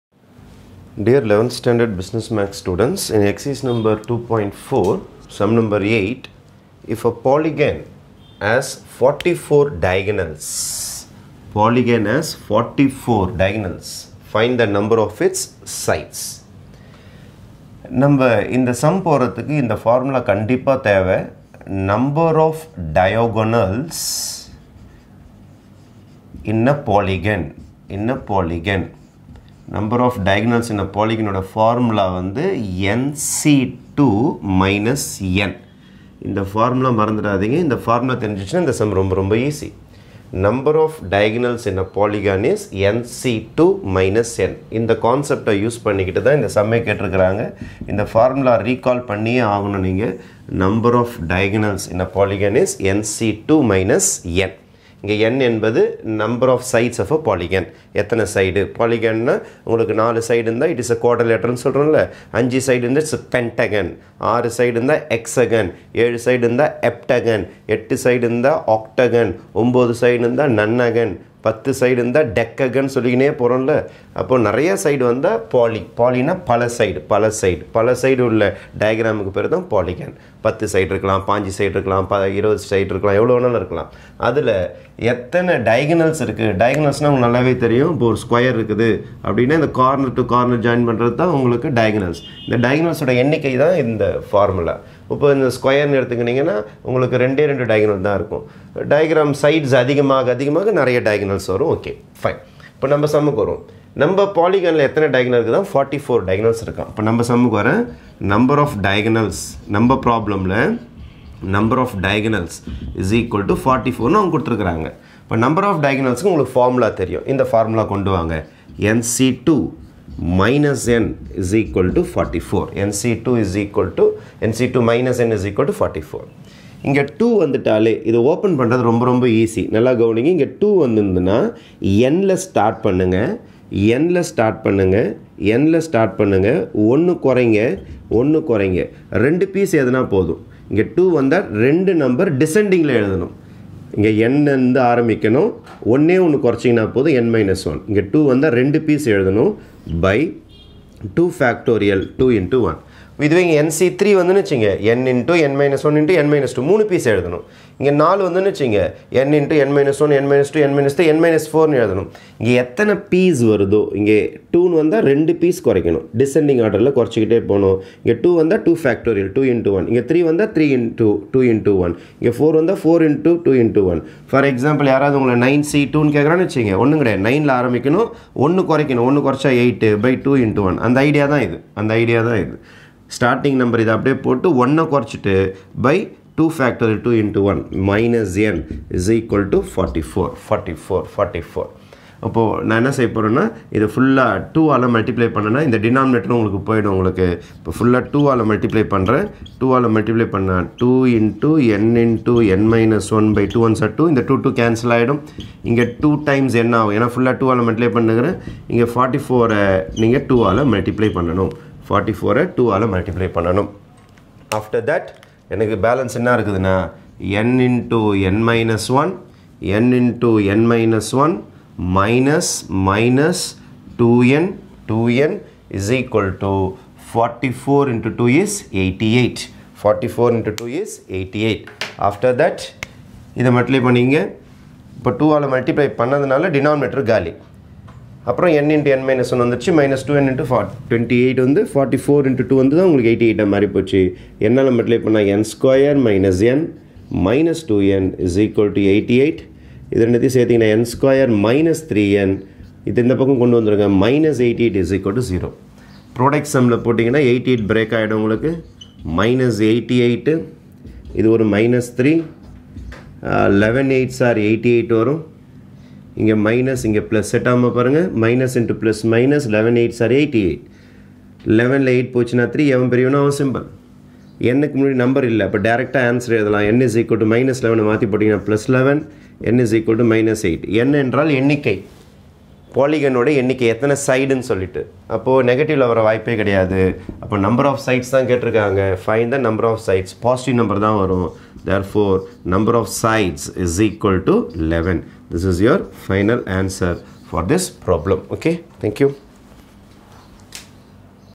11th Std BM Ex.2.4(8) If a polygon has 44 diagonals, find the number of its sides
TAMILNADU SYLLABUS 11th STD BUSINESS MATHS CHAPTER 2